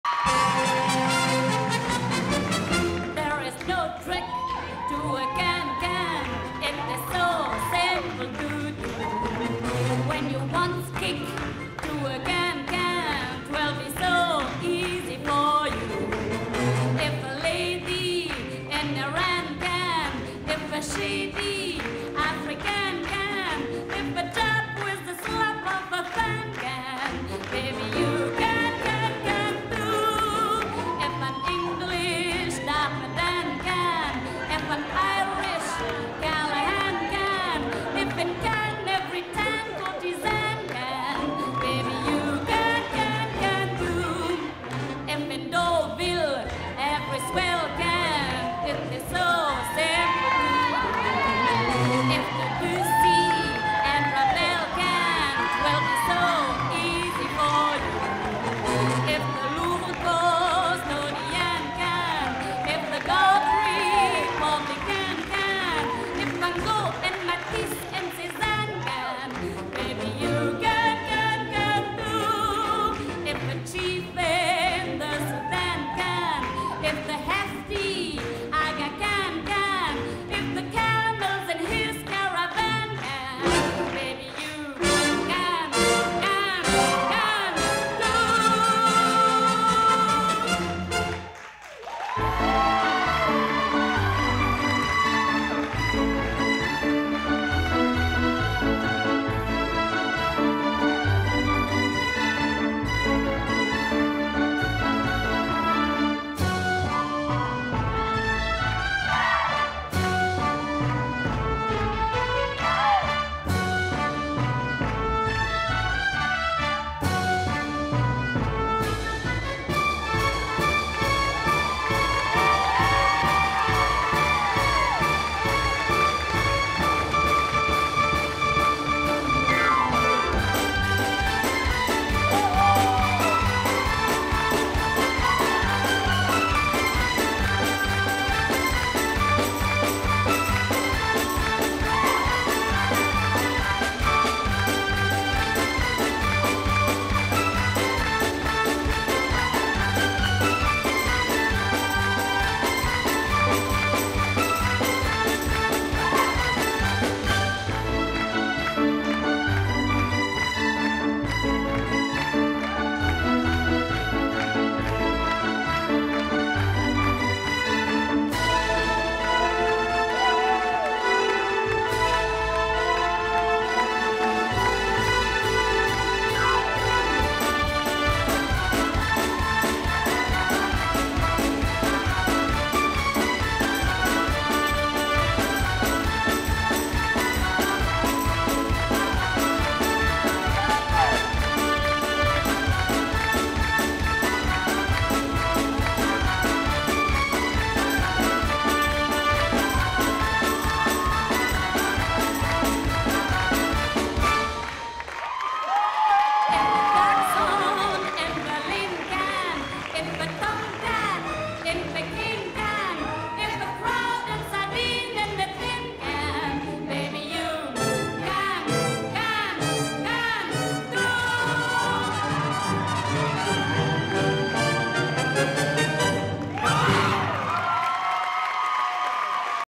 Ladies Can Can Formation
Ladies' Can Can Formation from Murder at the Moulin Rouge (March 22, 23, 29 and 3, 2019 Showcase (member of ASCAP)